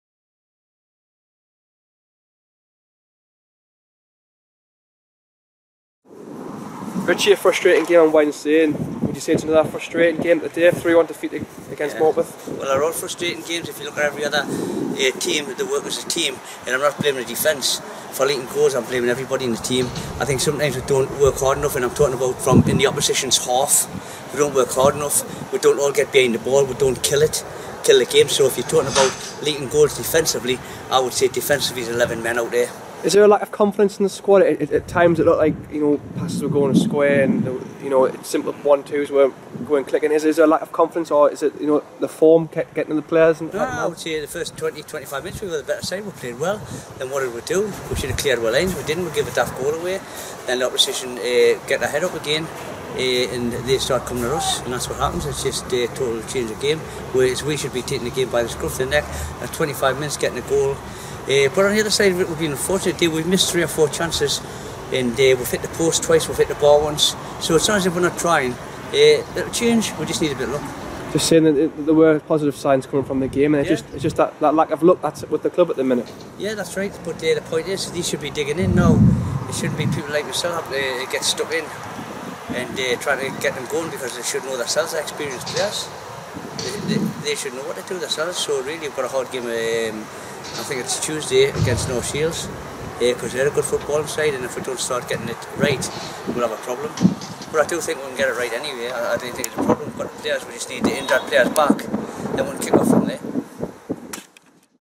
Roofing Reaction: 13/09/14
Here from Roofing Manager Richie McLoughlin after Saturday's game against Morpeth Town.